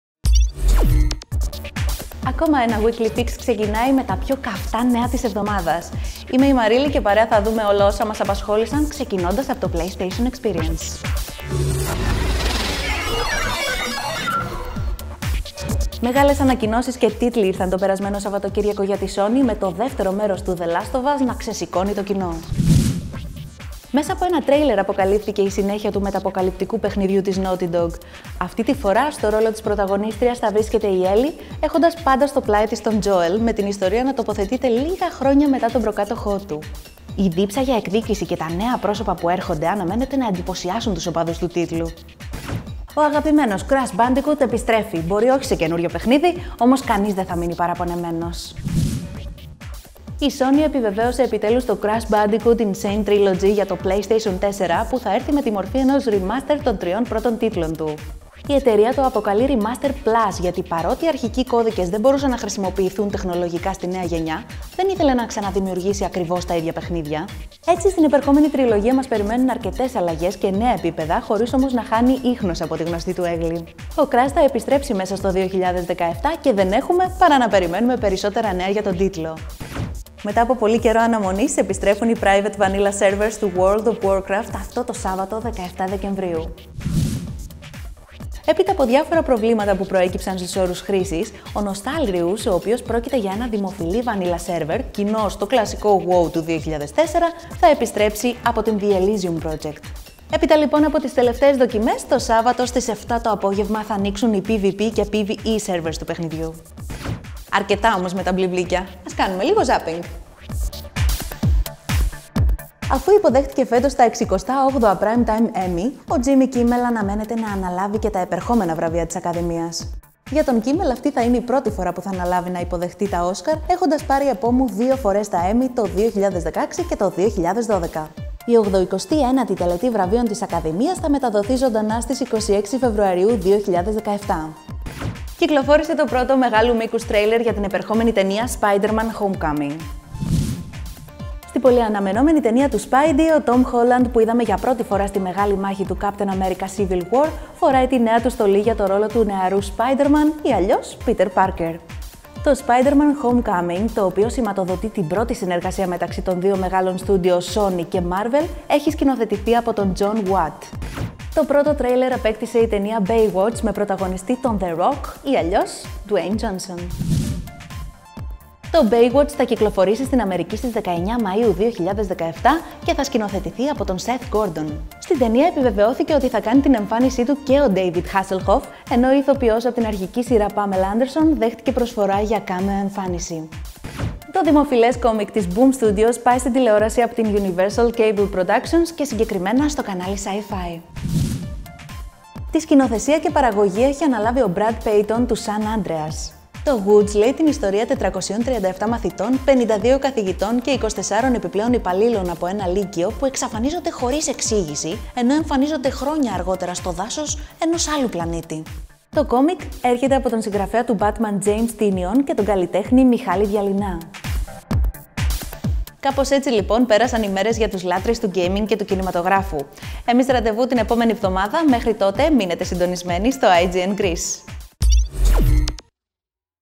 IGN Greece - Weekly Fix #44
Νέα παιχνίδια και νέα trailers ταινιών ήταν στα σημαντικότερα νέα της εβδομάδας που πέρασε.

Διαβάστε τα όλα αναλυτικά:

To The Woods στο SyFy